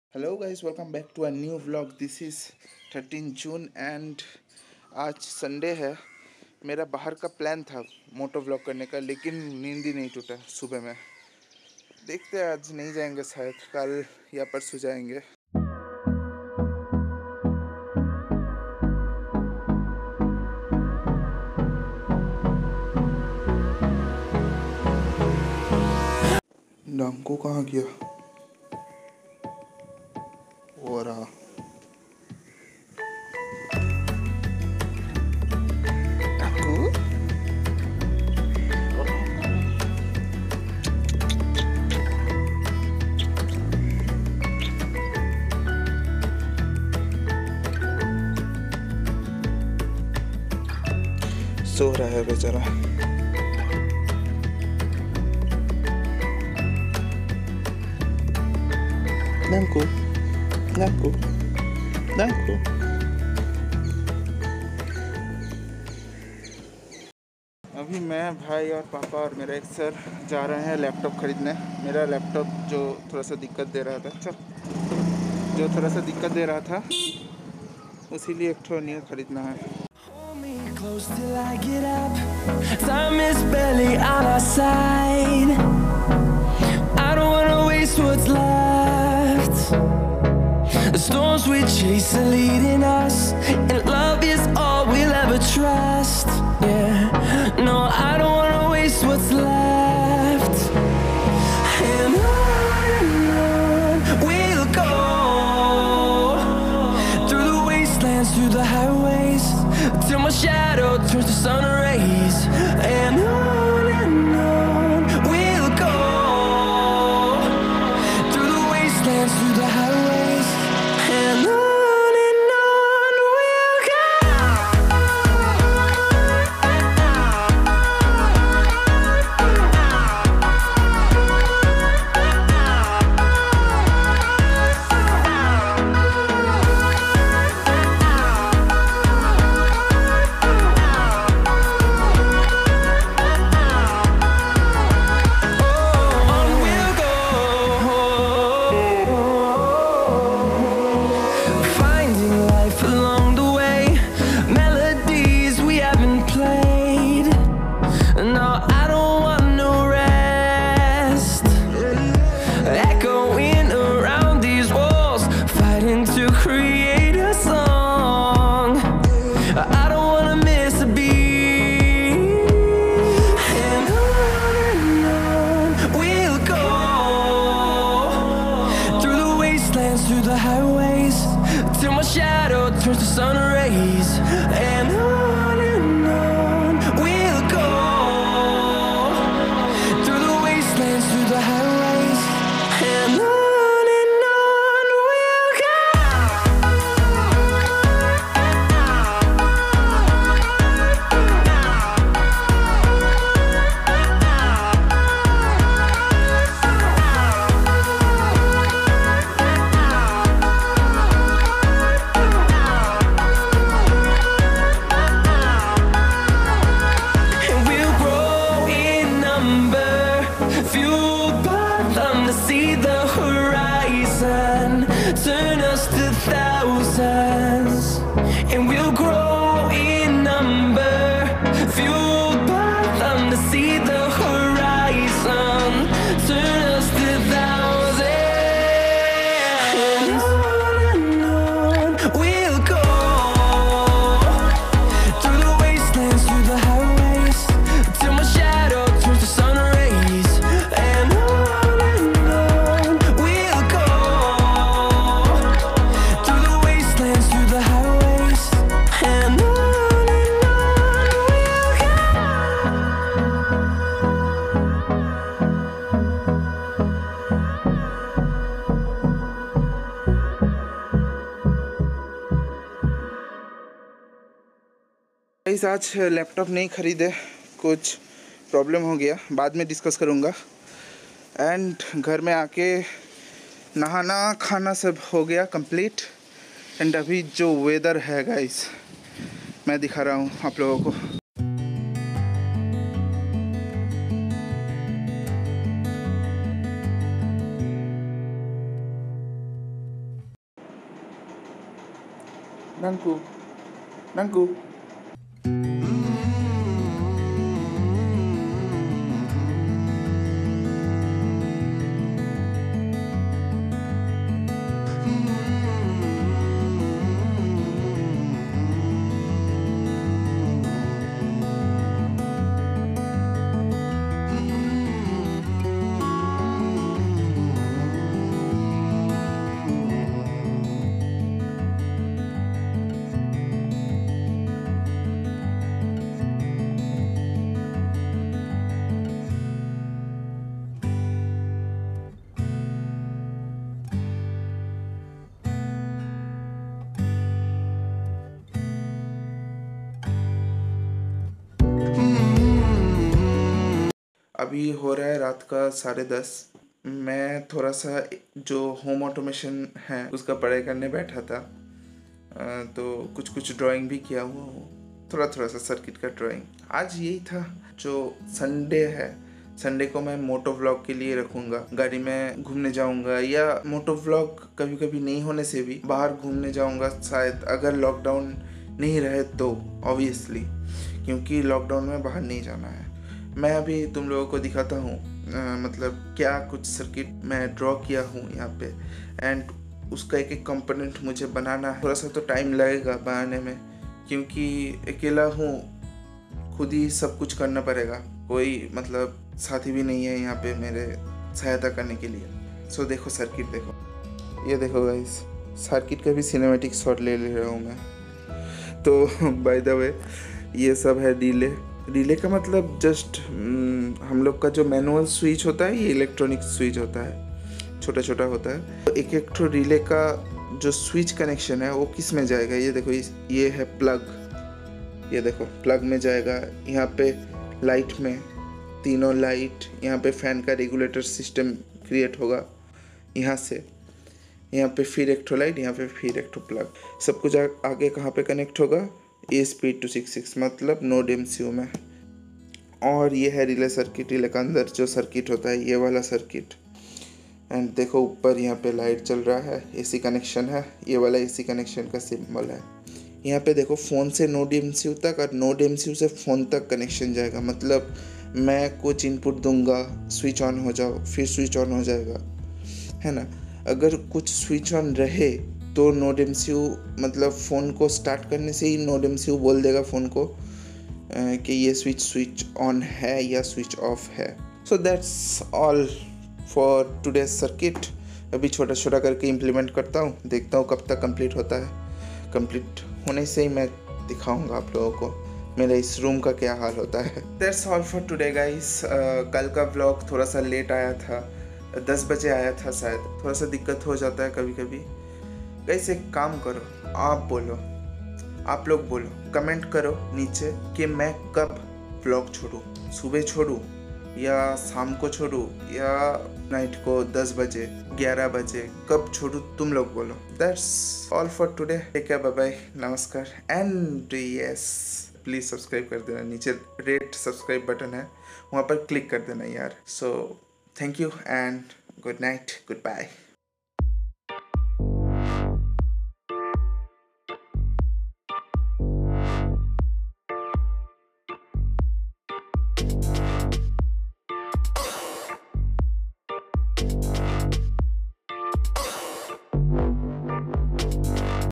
My First Moto-Vlog... || Aaj Ek Circuit Design Kie Hun... Vlog #19
Vlog #19

Hey guys, Aaj din var sab thik tha.. moto vlog kiye. and ghar mein aake ek circuit design kiya... I hope you all enjoy the video... And yea pls share it with your closer. Thanks to connect with Soft Connect 🥰.